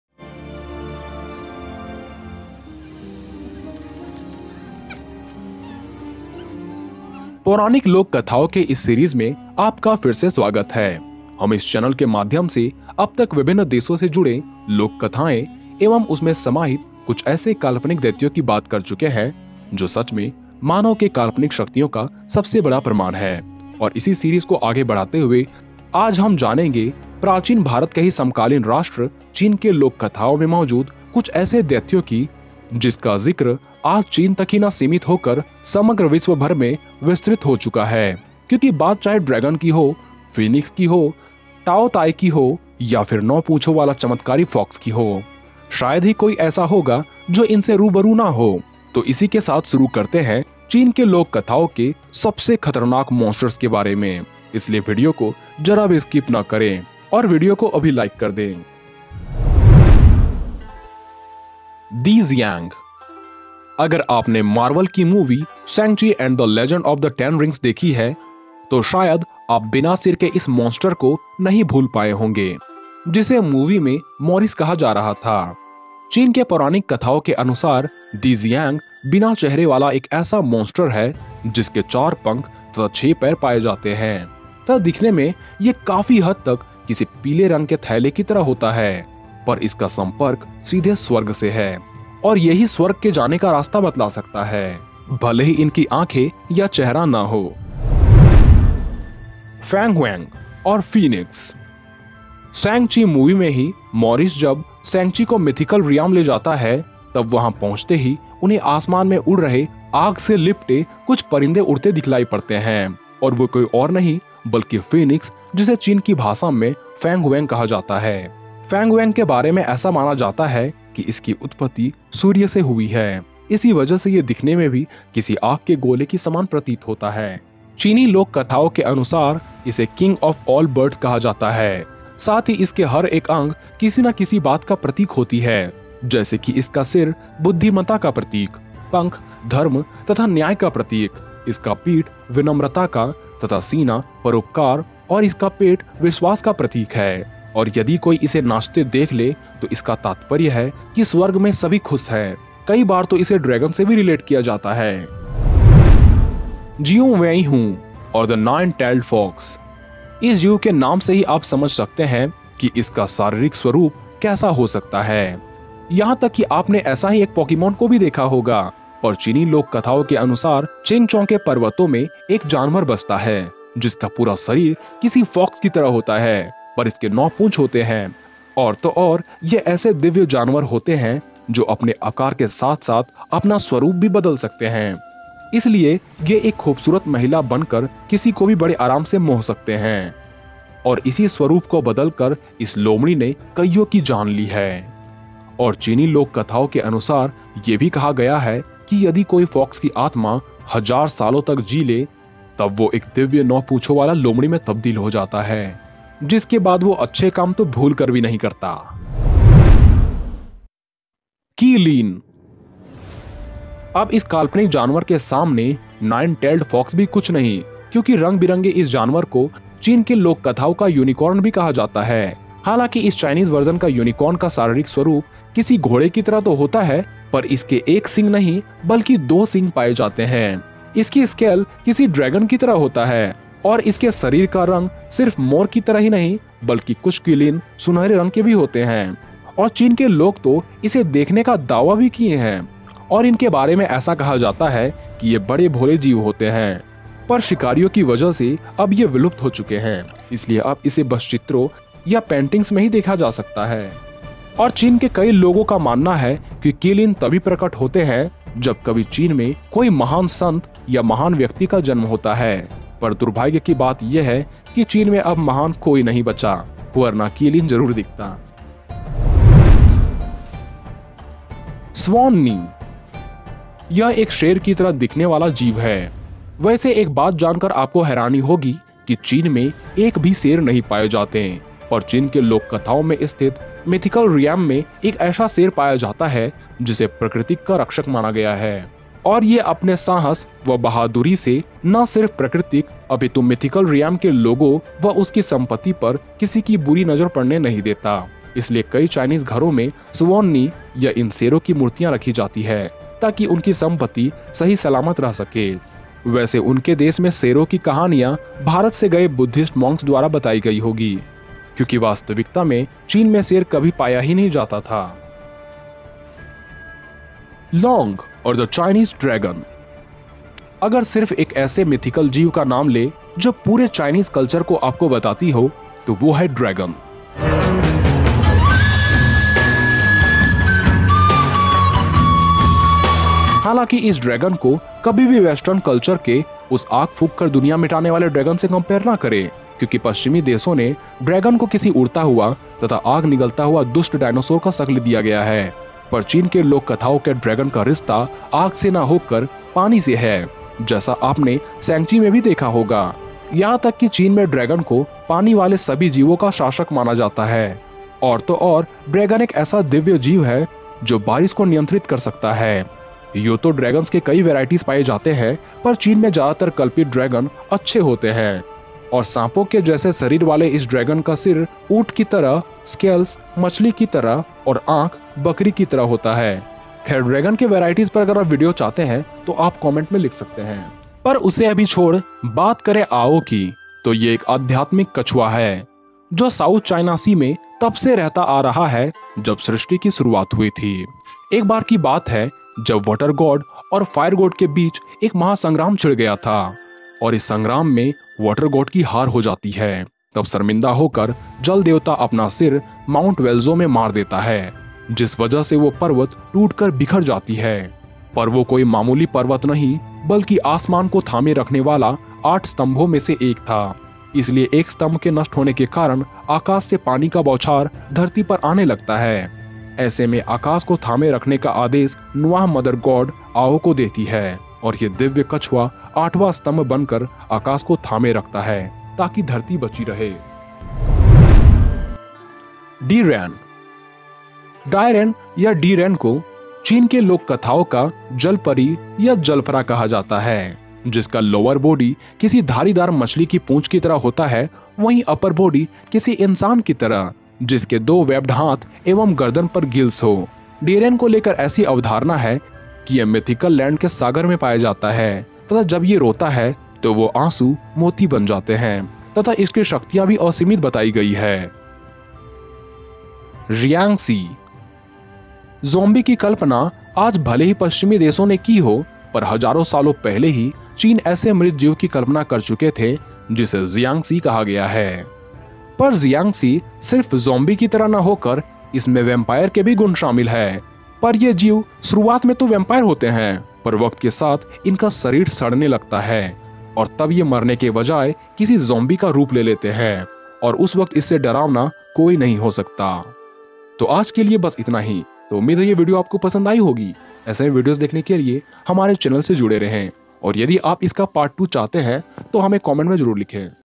चीन के लोककथाओं का सबसे खूंखार Monster | Chinese Mythological Creature
Chinese mythology (simplified Chinese: 中国神话; traditional Chinese: 中國神話; pinyin: Zhōngguó shénhuà) is mythology that has been passed down in oral form or recorded in literature throughout the area now known as Greater China. Chinese mythology encompasses a diverse array of myths derived from regional and cultural traditions. Populated with engaging narratives featuring extraordinary individuals and beings endowed with magical powers, these stories often unfold in fantastical mythological realms or historical epochs. Similar to numerous other mythologies, Chinese mythology has historically been regarded, at least partially, as a factual record of the past.